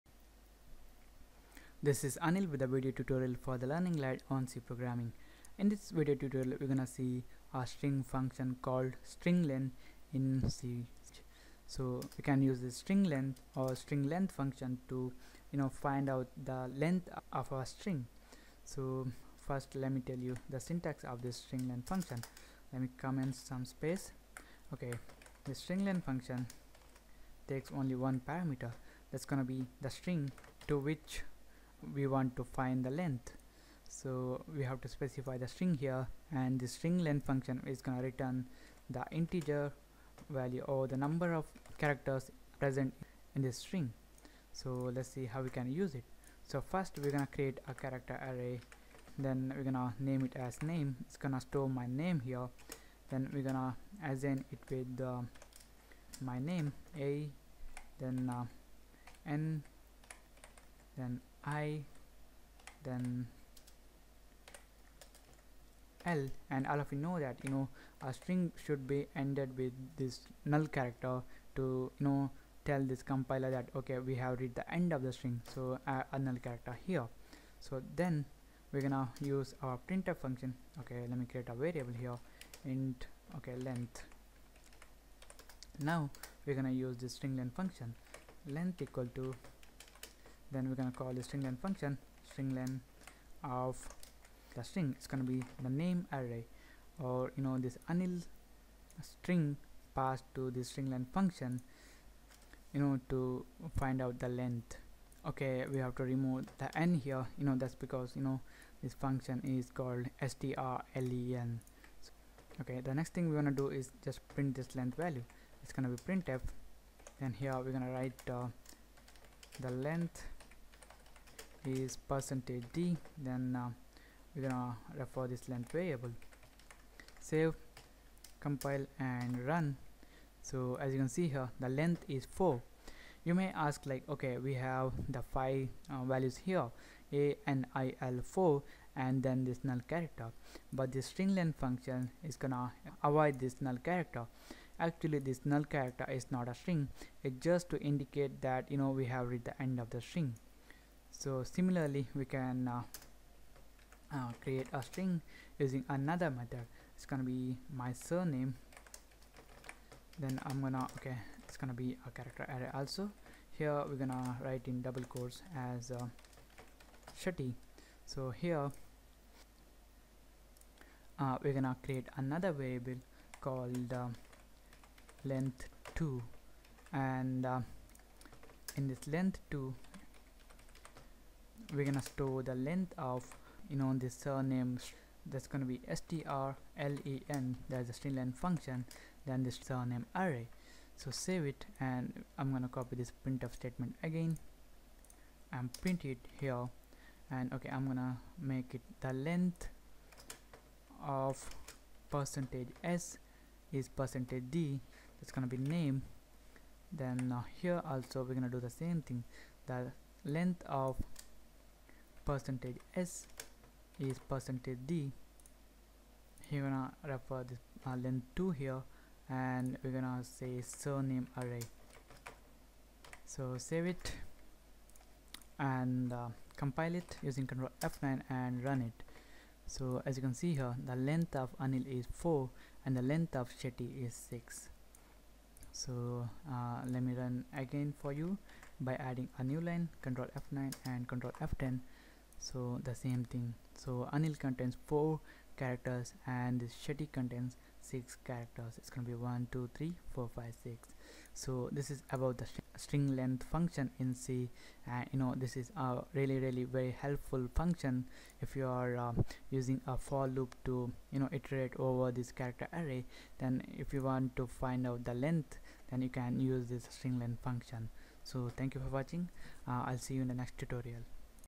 strlen String Length Function in C Programming Language Video Tutorial
In this C programming language video tutorial / lecture for beginners video series, you will learn how to get the length of a string using strlen() function in c in detail with example.

You will learn what is the use of strlen function, what is the syntax for using it, how it works what parameters / arguments it takes in detail with example.